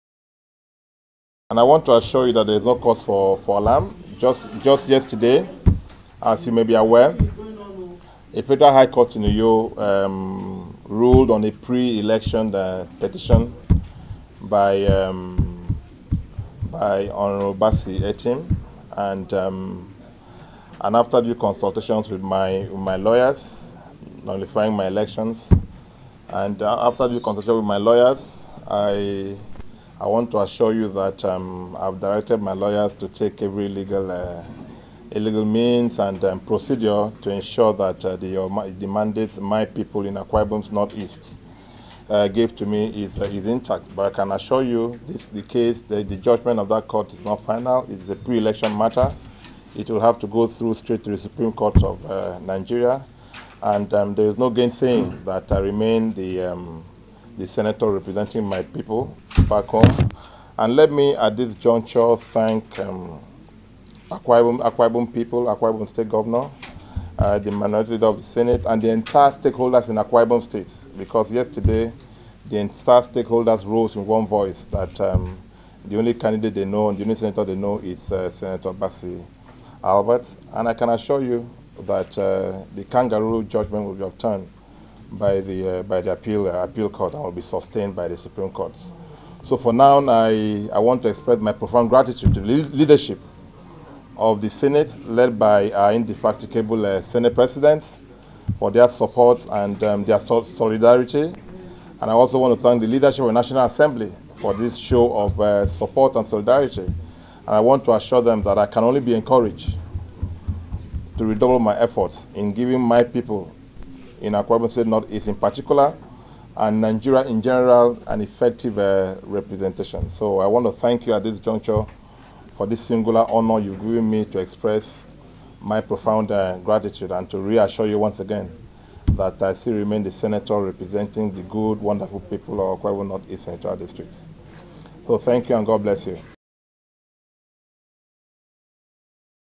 HIGH COURT COURT JUDGEMENT: Senator Albert Bassey Akpan calls for calm
The Senator representing Akwa Ibom North East, Bassey Albert Akpan has reacted to the judgement of Federal high court, sitting in Uyo, which nullified his election.

The verdict went in favour of Hon. Bassey Etim.

Briefing Senate Correspondent on Tuesday in Abuja, the Senator said the judgement was one which reflected “biased conduct of Justice Fatun Riman Fatun, throughout the trial of the case”.

The lawmaker pointed out that the matter was a pre election case, and that as such, he had directed his lawyers to exercise his rights of appeal, to ensure that justice at the court of appeal was served.

Senator Albert, who is also the Chairman of Senate Committee on Gas, disclosed that, his legal team had also filed an application for stay of execution of Justice Riman’s judgement, pending the hearing of the substantial appeal at the appellate court.

He, therefore, enjoined his supporters, constituents and well wishers to remain calm as he was still representing the people of Akwa Ibom North-East at the Senate chamber